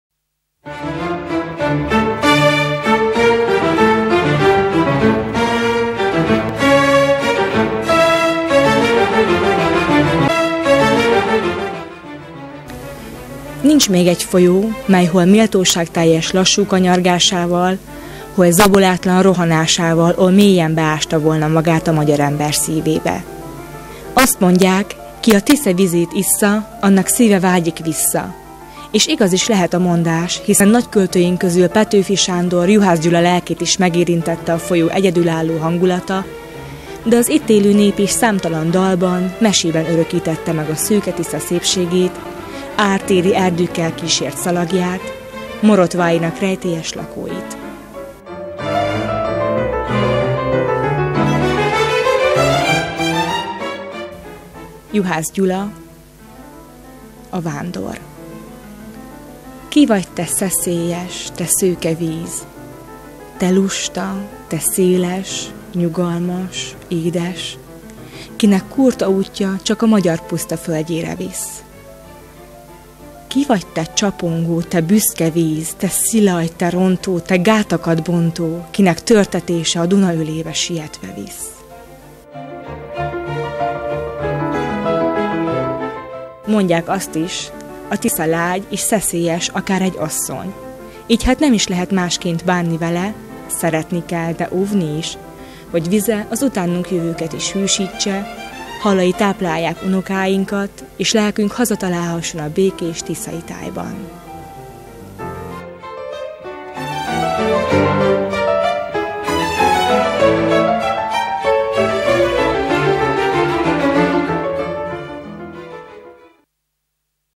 Írás-kép - Juhász Gyula: A vándor /2007.02.09./
Írás-kép – verses etűd
Juhász Gyula: A vándor című versét elmondja: Horváth Ildikó